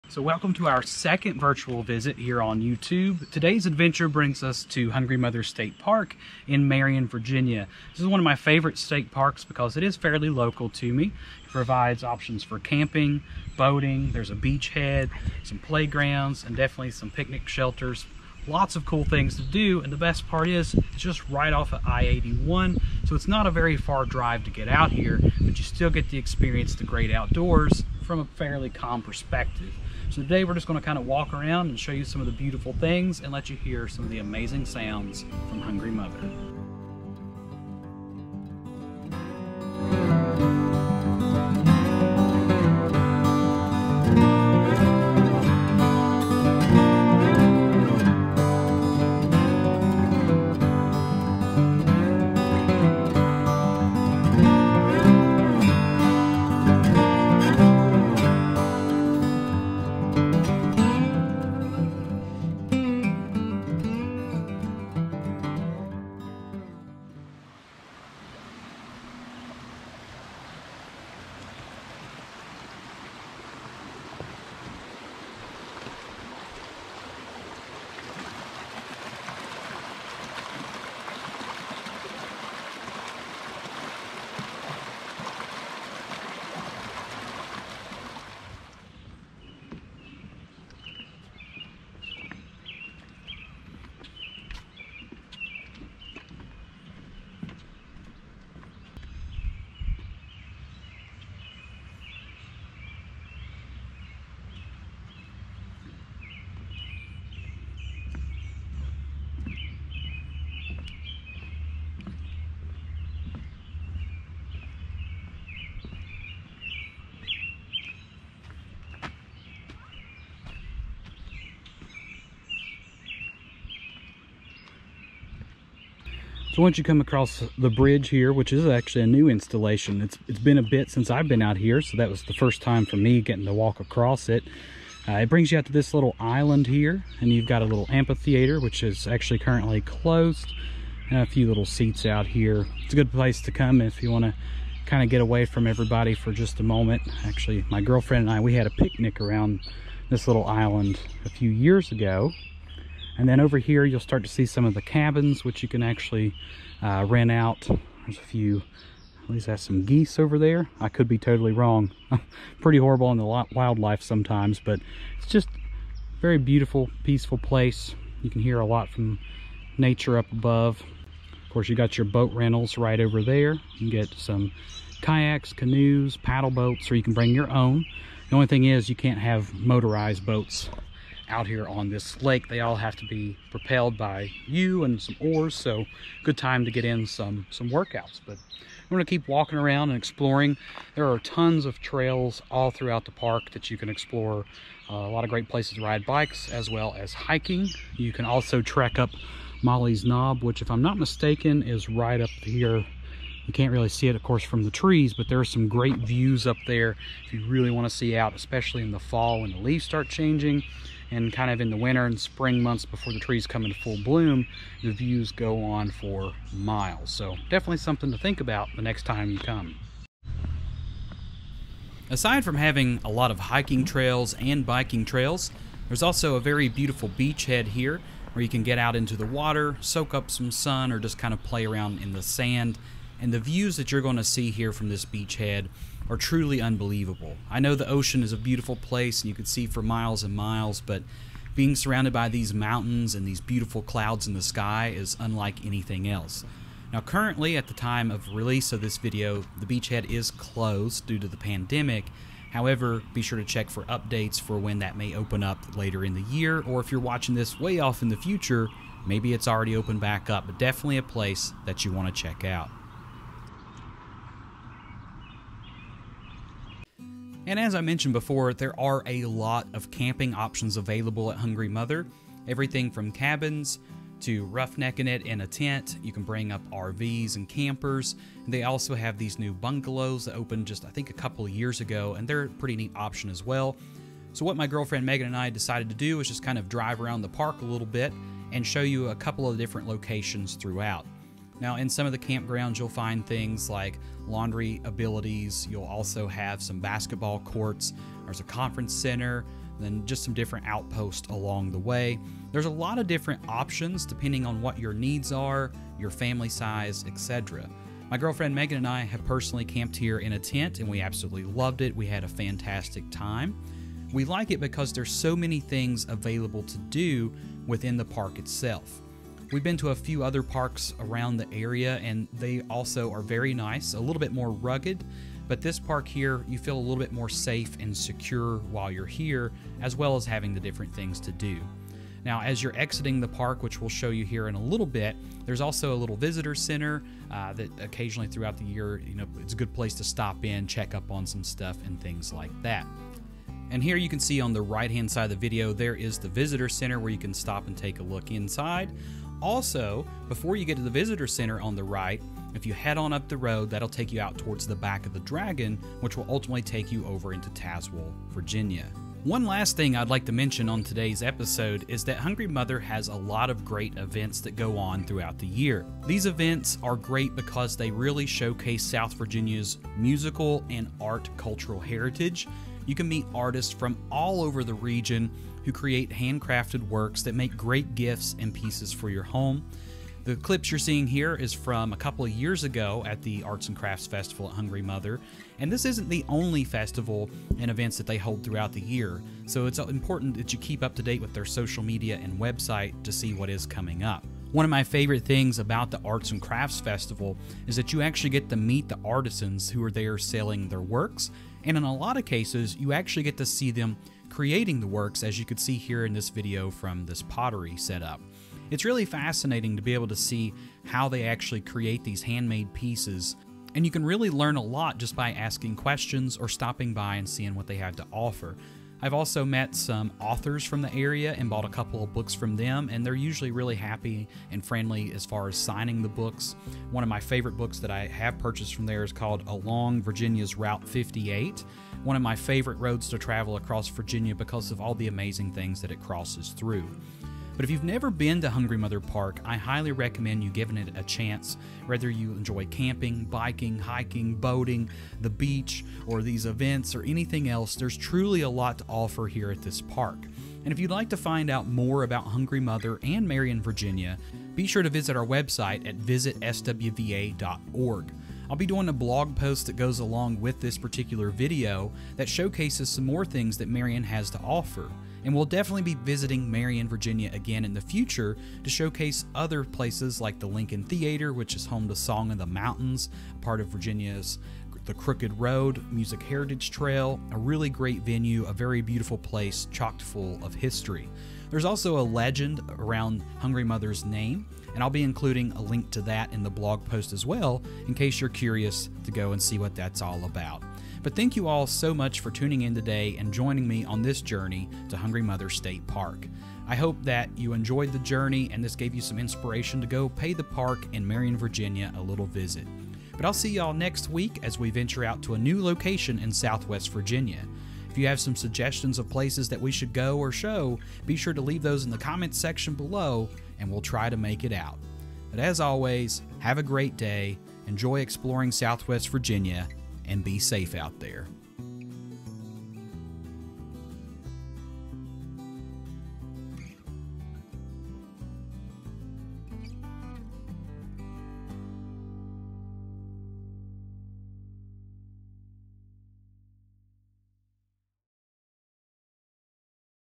SWVA Virtual Visit - Hungry Mother State Park, Marion Virginia
Today's virtual visit takes us to Hungry Mother State Park in "America's Coolest Hometown" Marion, Virginia.

Hungry Mother State Park is a fantastic Virginia State Park located just fifteen minutes off I-81 in Marion, Virginia. Hungry Mother offers a wide variety of outdoor fun from camping, biking, hiking, picnics, boating, a beach, and much more. That park is also located right along with The Back of the Dragon roadway leading over to Tazewell, Virginia.